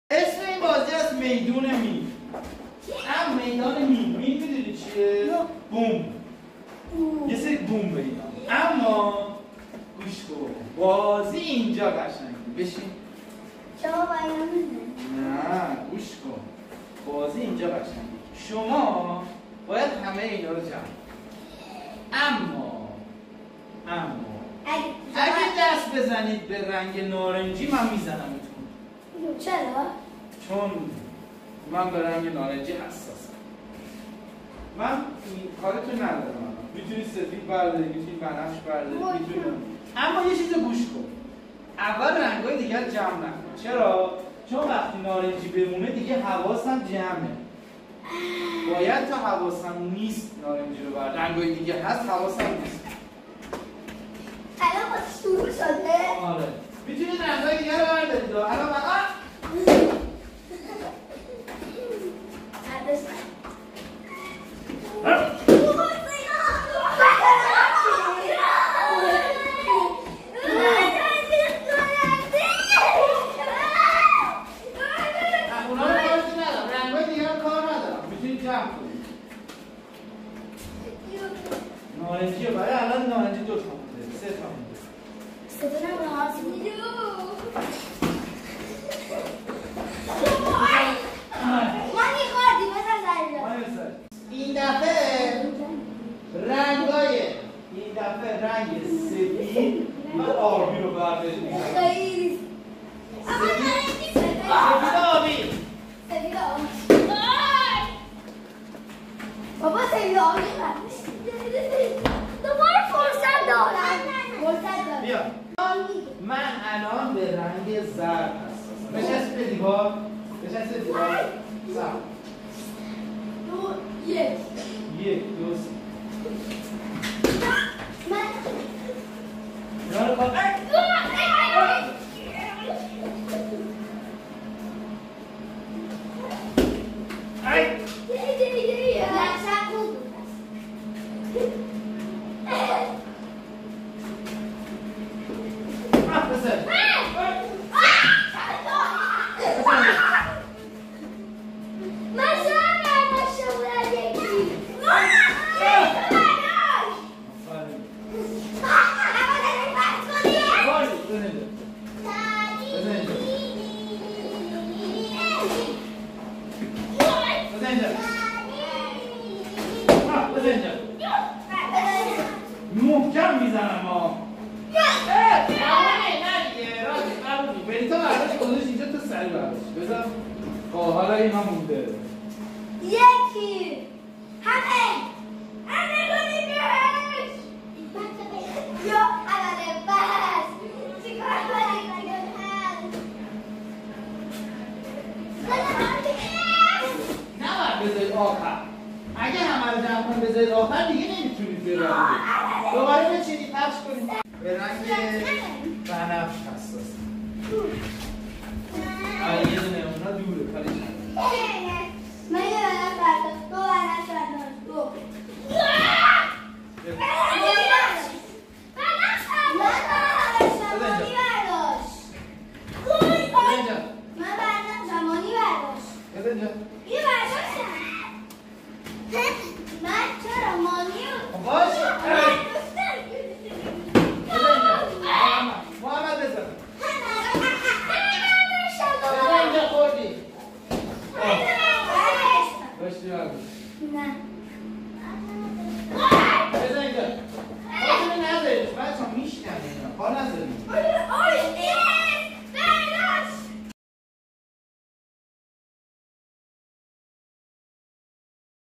P.E game for kindergarten | Color Bomb
🎯game name: Color Bomb
⛳Game goals: improving kids' reaction time and coordination
🎖how to play this game: In this physical education game, children have to collect bombs with a special color, but the coach is sensitive to color. If the children pick that color, the coach will look for them.